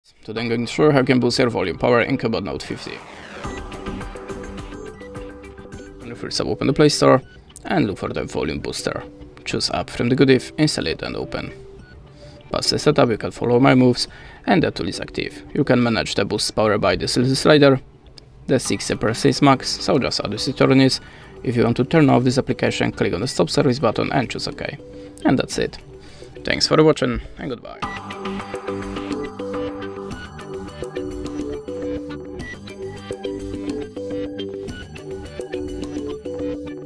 How to Boost Volume Power on Cubot Note 50?
Find out more:
Unlock the full potential of your audio output on the Cubot Note 50 by learning how to boost volume power. Whether you're watching videos, listening to music, or engaging in hands-free calls, having adequate volume levels is essential for an immersive experience. In this tutorial, we'll explore various methods to increase the volume power on your Cubot Note 50, allowing you to enjoy your favorite media content at optimal levels without compromising quality.

Why would I want to boost the volume power on my Cubot Note 50?
Are there any limitations or restrictions on maximum volume levels imposed by the device?
Where can I find the volume settings on my Cubot Note 50?
Can I adjust volume levels for different types of audio output, such as media playback, calls, and notifications?
What options are available for boosting volume power on the Cubot Note 50?